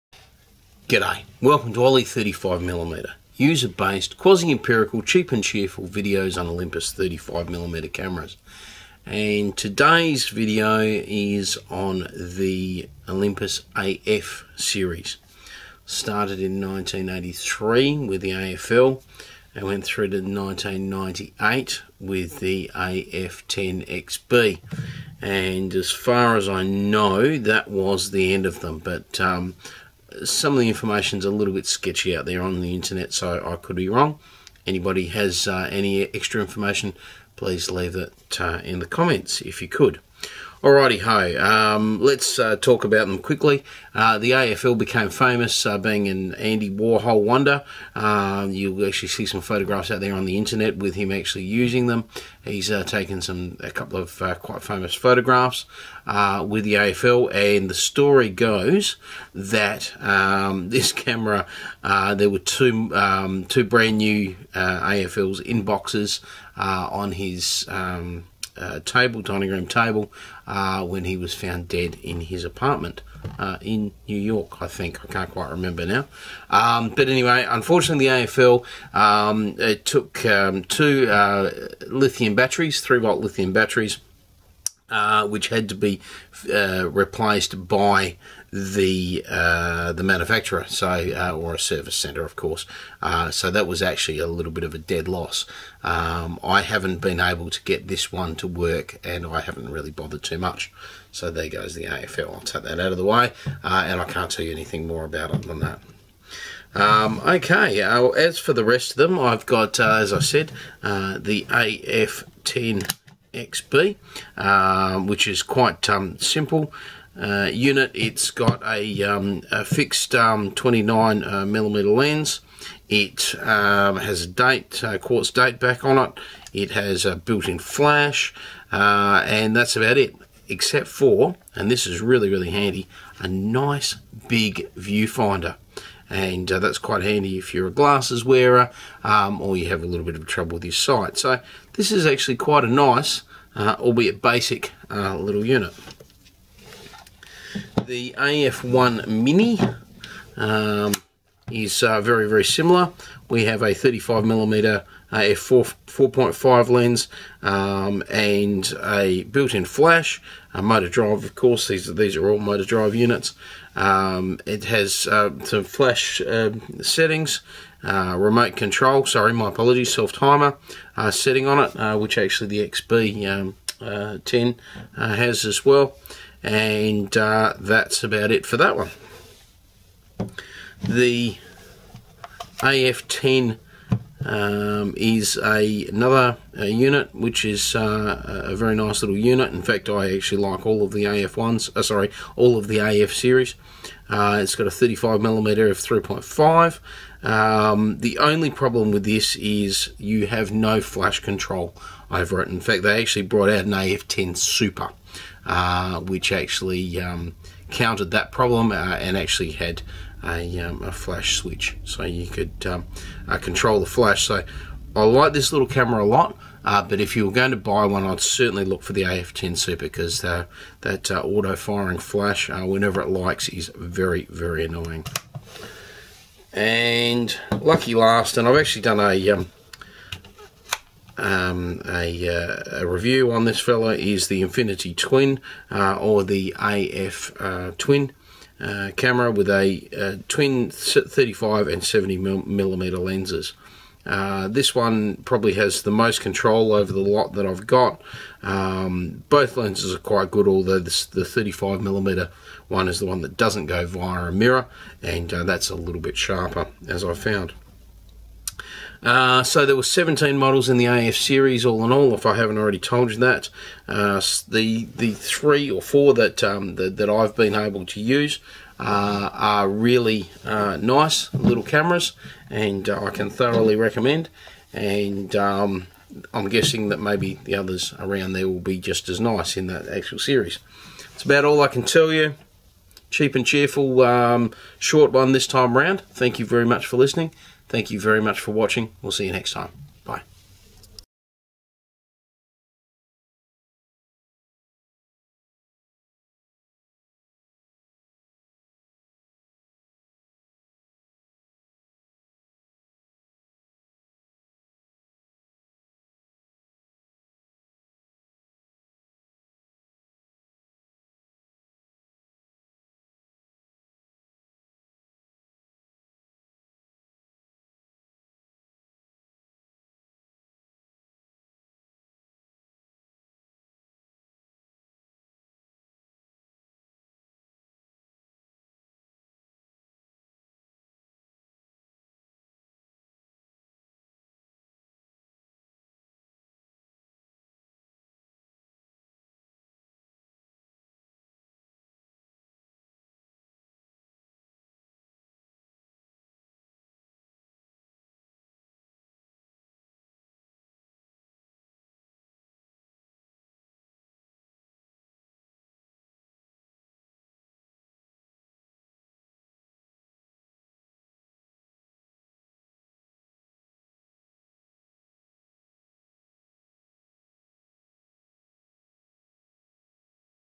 Oly35mm Overview - AF Series
A loose collection of fixed lens AF models, starting with the AFL (Japanese: Picasso) in 1983, through to the AF-10 XB in 1998. They seemed to all be reasonable cameras with no real exciting features or innovations, with the possible exception of the twin lens AF-1 Twin (US: Infinity Twin).